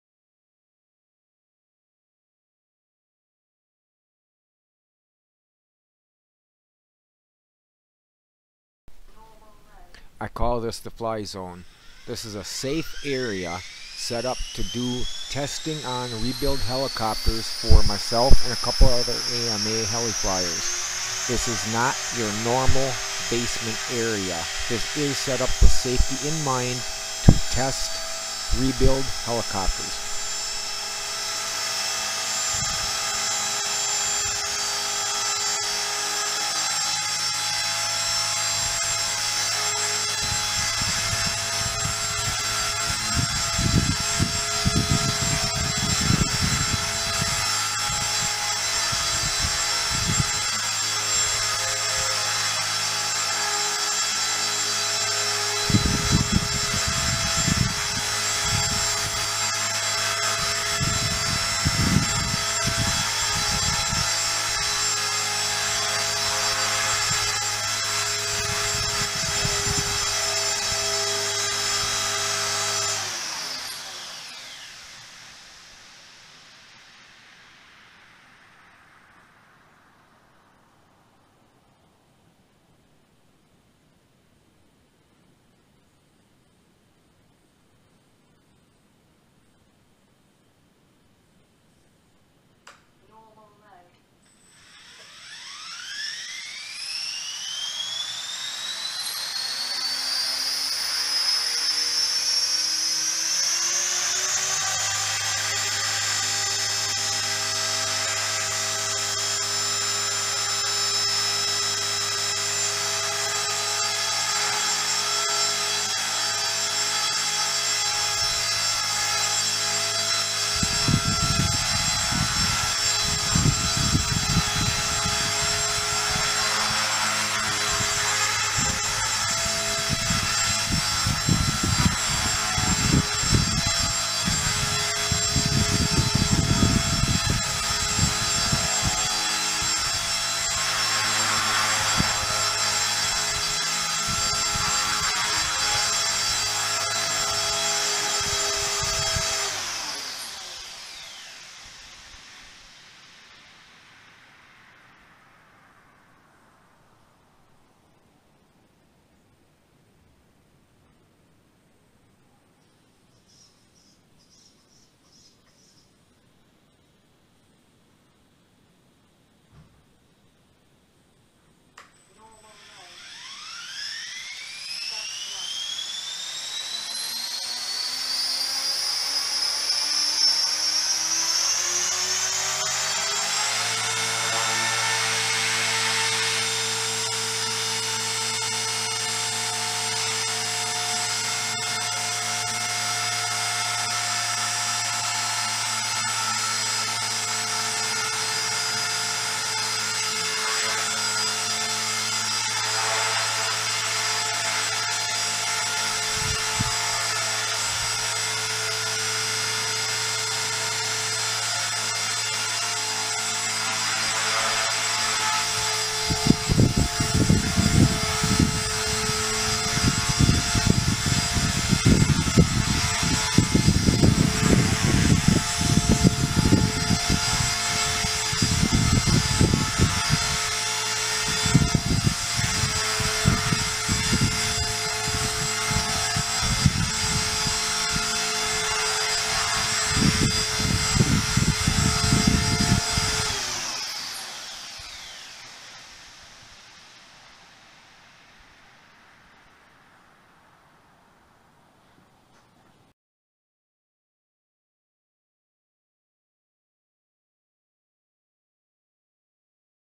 Oxy 3 Sport testing
I do not condone flying inside other than a gym for anything bigger than a micro (130 and below) .  This is being done in a  area set up as a test zone that a few of us club members use to test rebuild build heli's for any vib's pre going to the field.  Tests are done in stages also for safety.  ALL Blades off for the first tests so we know a heli will not be freaking out and totally out of control. NOTHING bigger than a 450 will be test hovered.